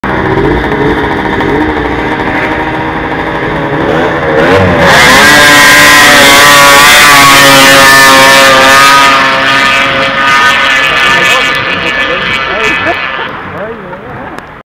acceleration St-Rose 18
quelque course motoneige a ST-Rose de Poularie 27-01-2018